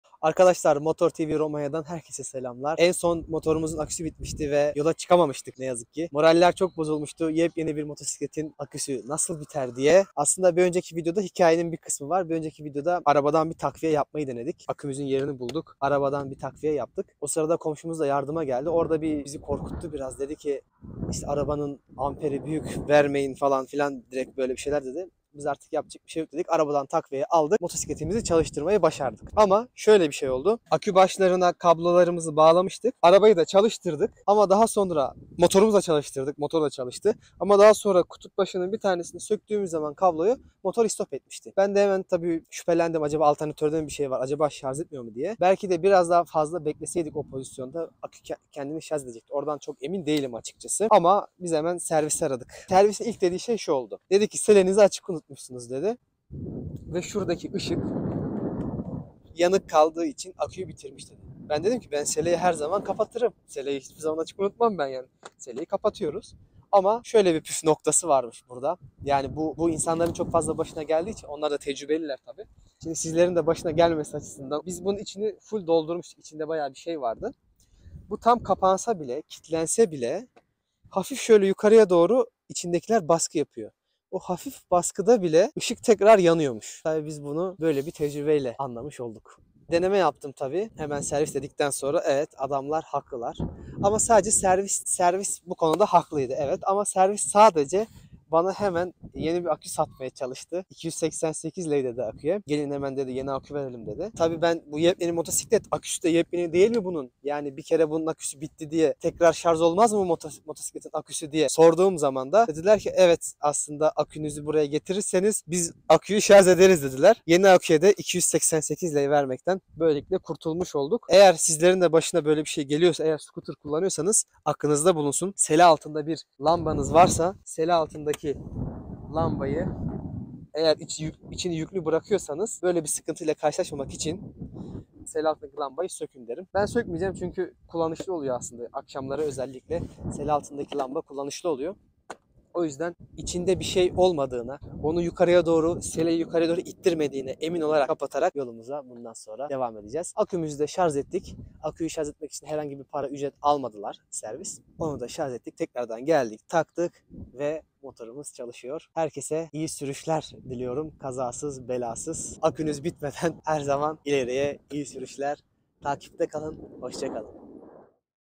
Aküyü Bitiren Hata! Siz Bu Hataya Düşmeyin
Selenin içindekiler seleyi yukarı doğru ittirdiği için sele altındaki lamba açık kalmış ve akümüz bitmiş. Aman dikkat, siz bu hataya düşmeyin! İyi seyirler!